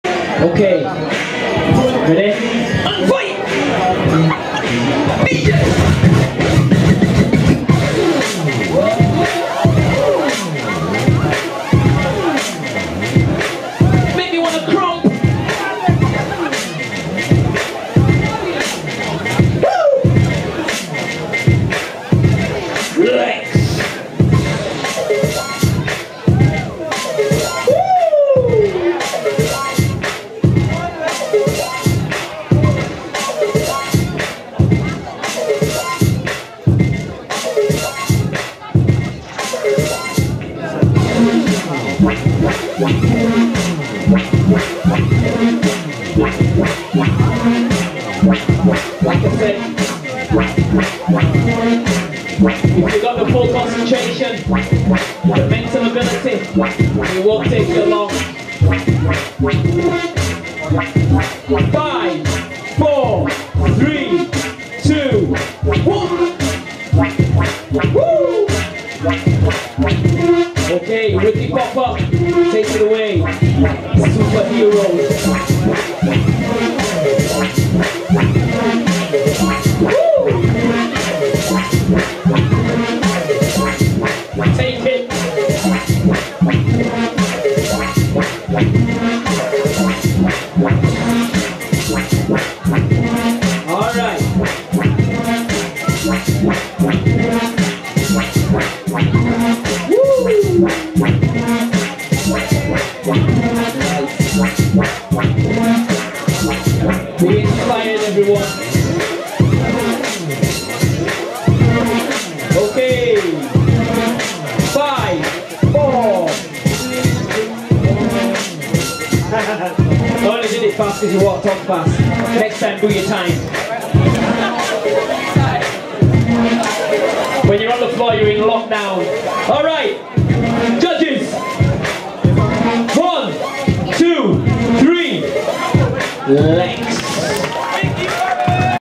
FSTV l FF Aug 12 l Popping l Top 16 l Ricky Popper vs Lex
Event: Fresh Factory Saturday 25th August 2012
Location: St Patrick Center, Huddersfield, UK